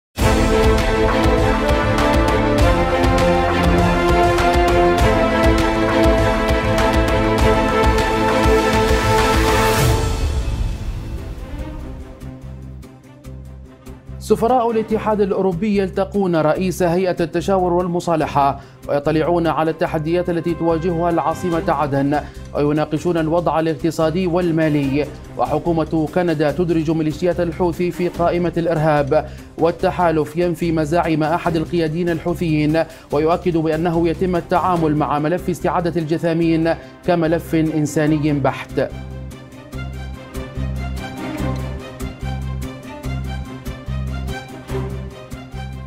مقدمة المحور_السياسي من نشرة أخبار قناة_المكلا 3 ديسمبر 2024م
سفراء الاتحاد الأوروبي يلتقون رئيس هيئه التشاور والمصالحة ويطلعون على التحديات التي تواجهها العاصمة عدن، ويناقشون الوضع الاقتصادي والمالي، وحكومة كندا تدرج مليشيات الحوثي في قائمة الارهاب، والتحالف ينفي مزاعم أحد القياديين الحوثيين، ويؤكد بأن التعامل مع ملف استعادة الجثامين كملف إنساني بحت.